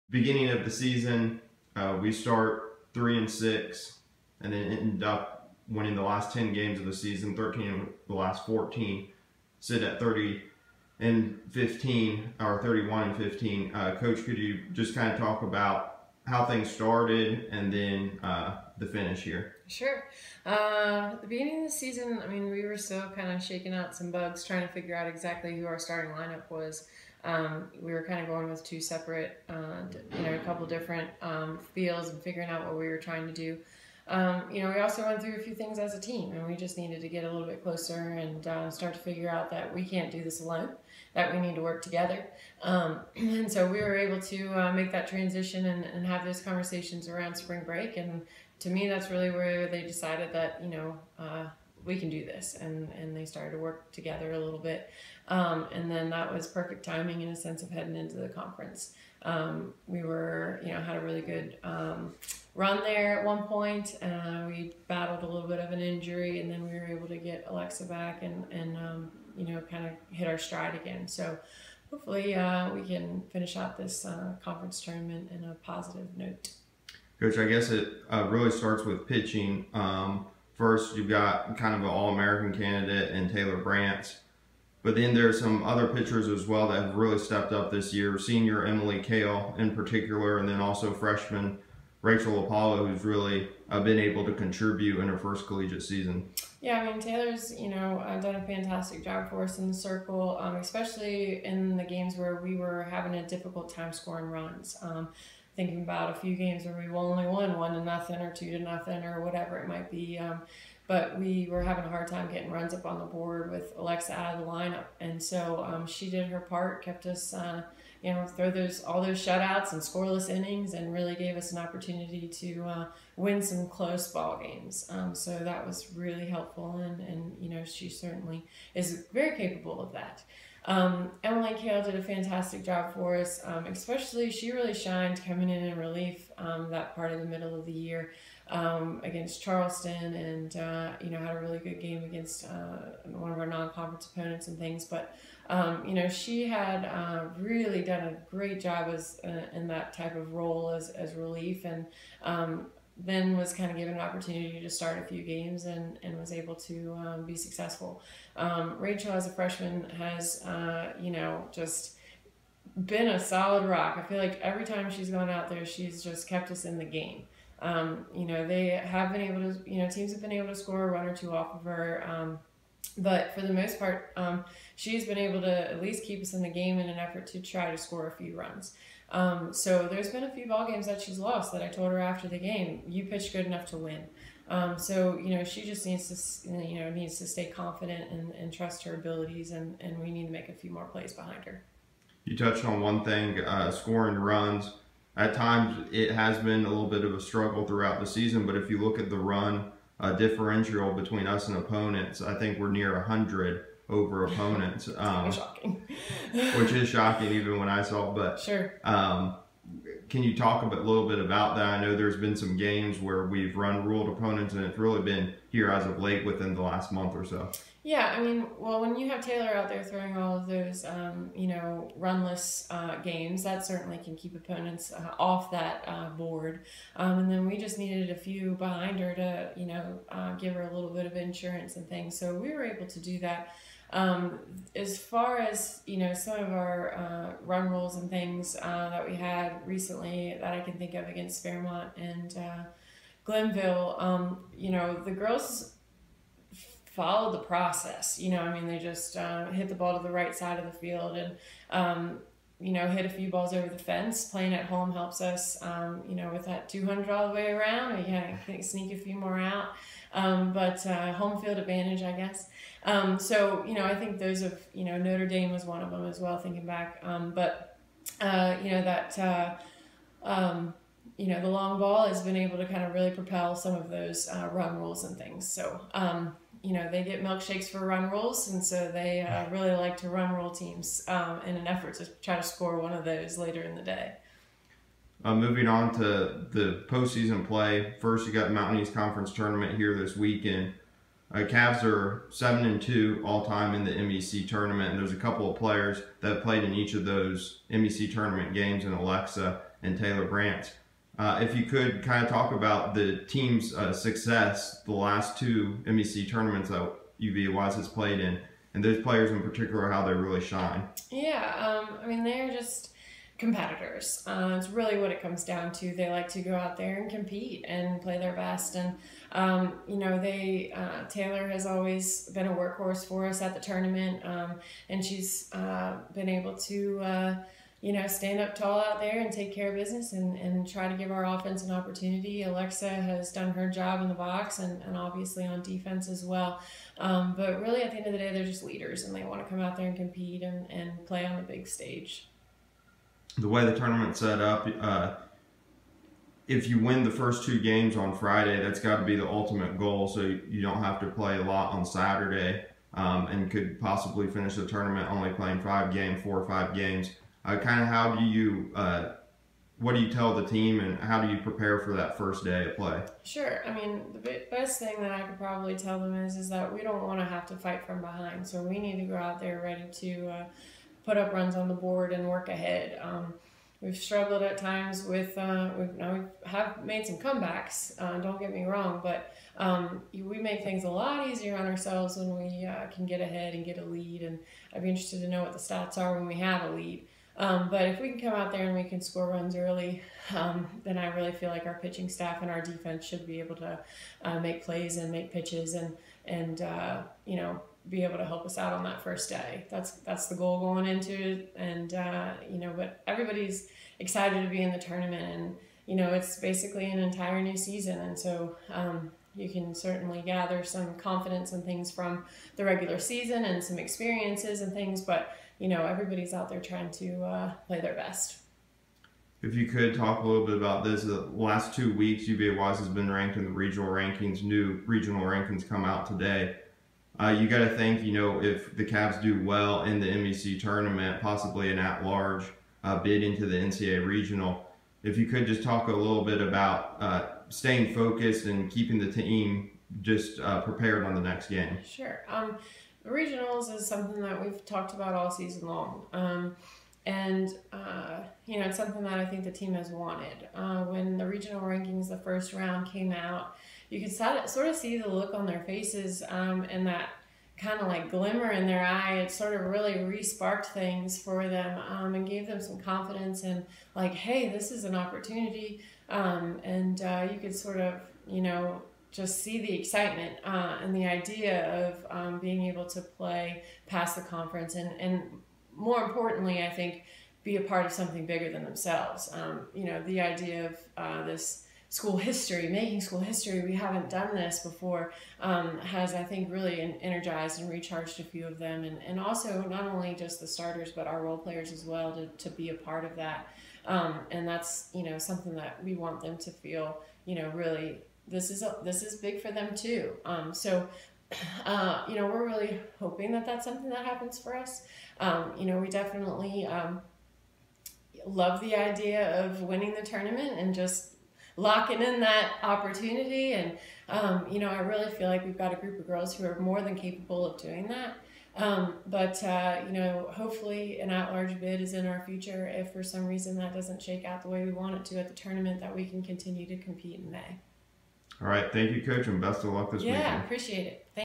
Softball Co-Head Coach Karen Bitter Previews the 2017 MEC Tournament
UVa-Wise softball co-head coach Karen Bitter recaps the regular season and previews the 2017 Mountain East Conference Softball Tournament.